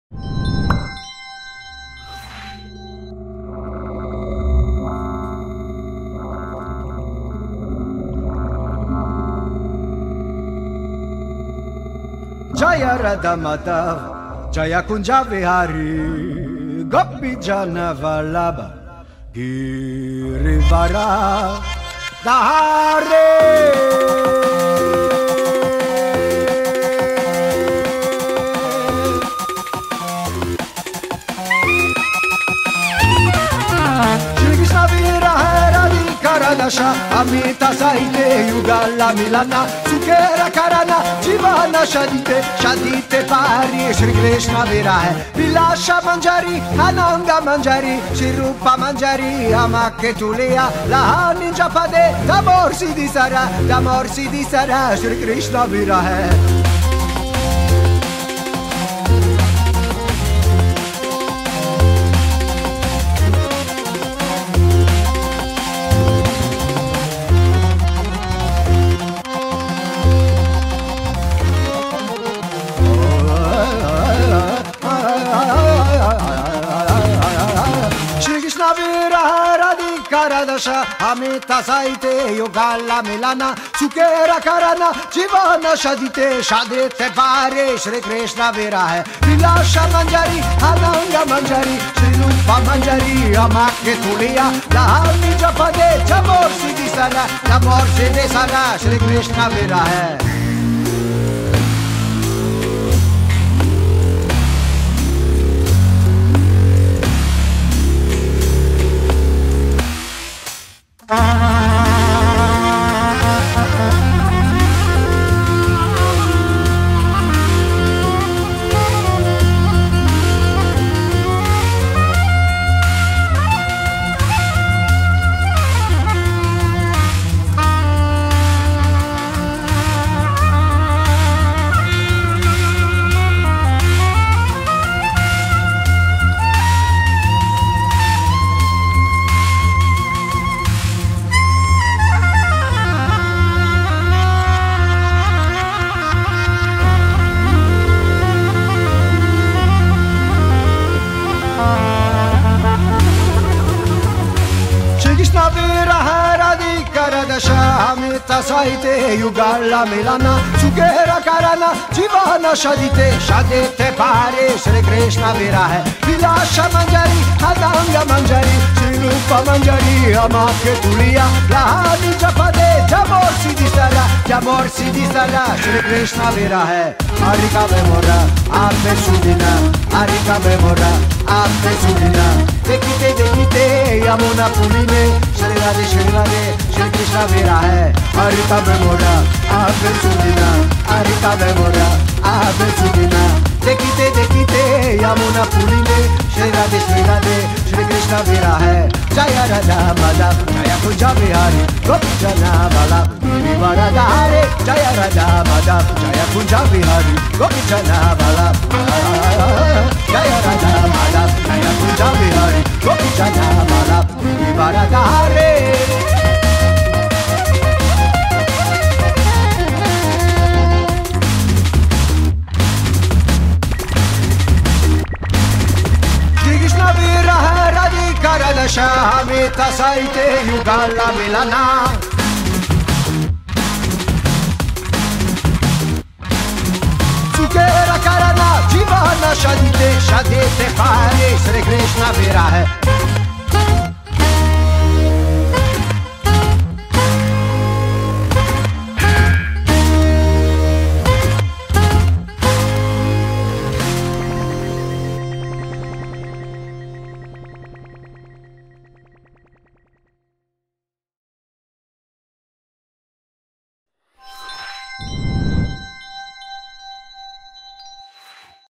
HOLLSESSION #12 - Olivero & SUKH MAHAL
Olivero & SUKH MAHAL viennent enflammer notre rentrée !

Encore une collaboration fructueuse avec nos amis de KiéKi Musiques, qui nous permet de vous présenter ce groupe atypique, combo entre musiques indo-pakistannaises et drum'n bass/électro..

Une fusion originale, portée par Olivier Olivero à l'harmonium, Sébastien Lauquère à la basse et Marc Mouchès au saxophone.

Une belle rencontre qui, nous l’espérons, vous plaira aussi ;)